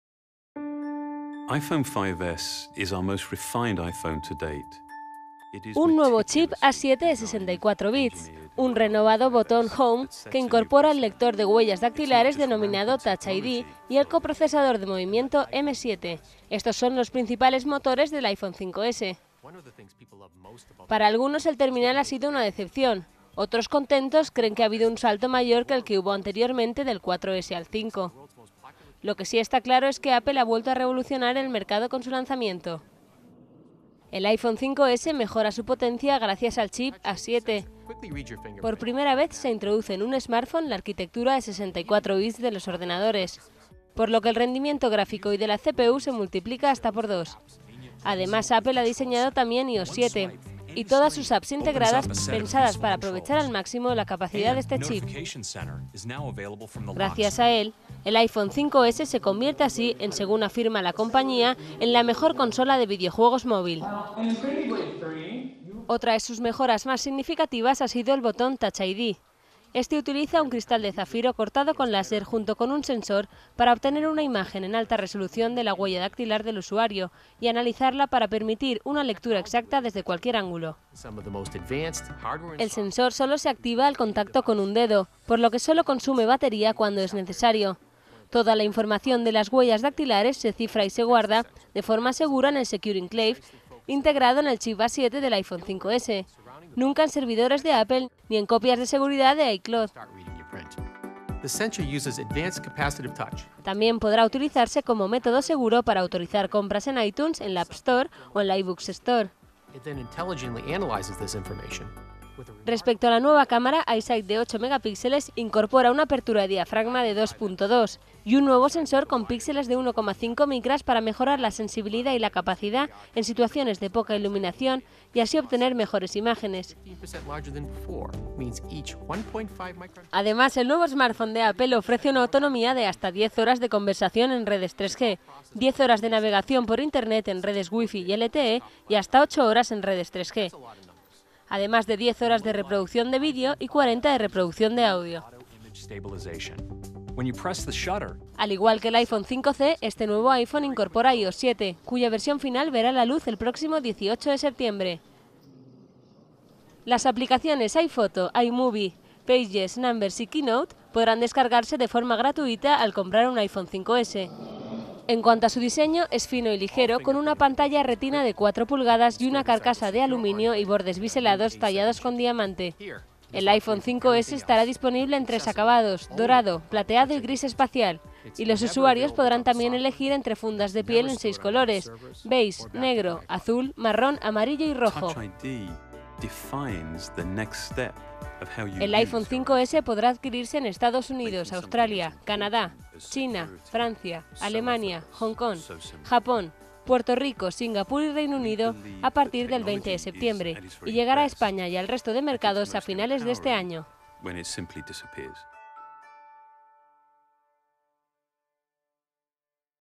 Touch ID, chip A7 y coprocesador M7 lo mejor del iPhone 5S
Apple ha presentado este martes el iPhone 5S, un nuevo modelo de su 'smartphone' que cuenta con un nuevo chip A7 de 64 bits. además, el nuevo terminal cuenta con un renovado botón Home que incorpora un lector de huellas dactilares denominado Touch ID, que permite desbloquear el teléfono simplemente tocando con un dedo. El nuevo iPhone 5S estará disponible en negro, blanco y dorado a partir del 20 de septiembre en algunos mercados.